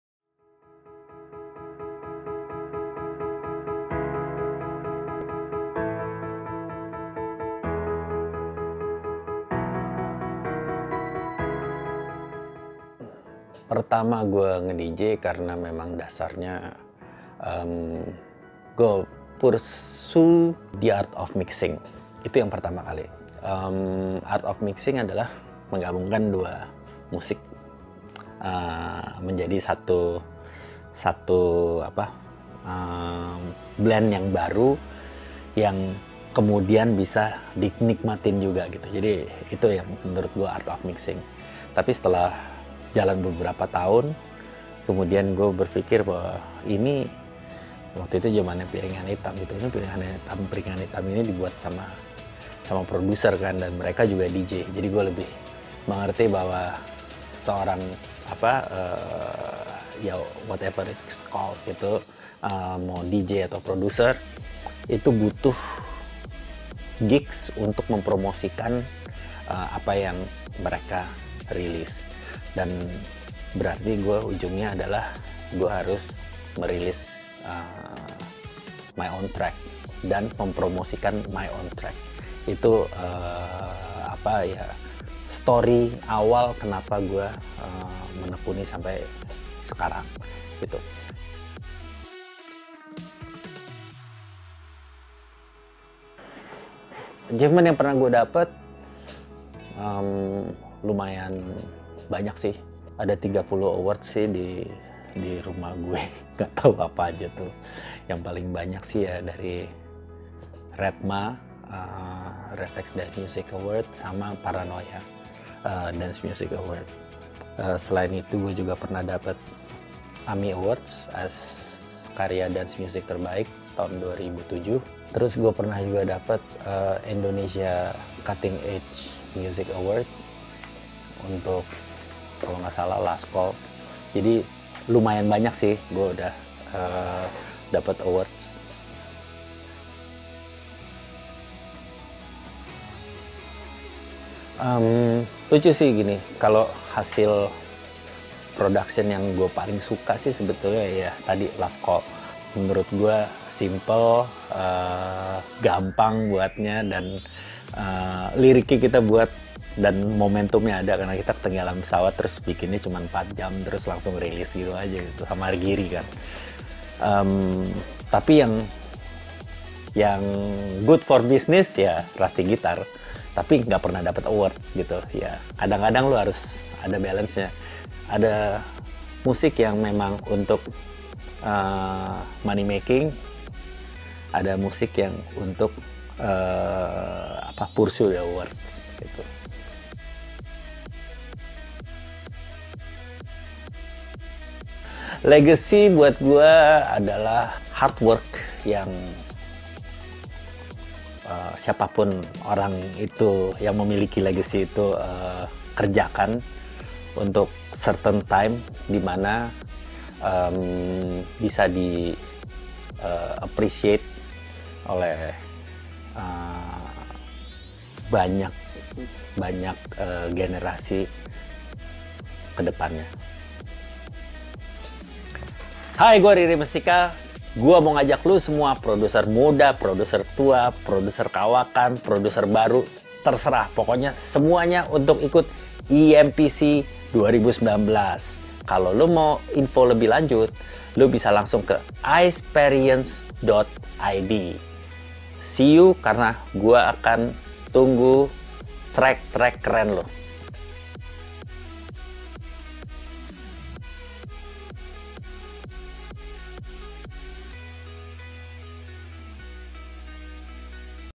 EMPC - Interview with Riri Mestica
The Founder of Spinach Record, Riri Mestica saying hi to you. Don’t miss the chance and submit your original music to join EMPC before the submission period ends!

Cuma di LAzone.id, ngeliat sesuatu dari sudut pandang yang beda, mulai dari musik, film, sampai lifestyle, karena yang sama nggak ngebawa lo ke mana-mana!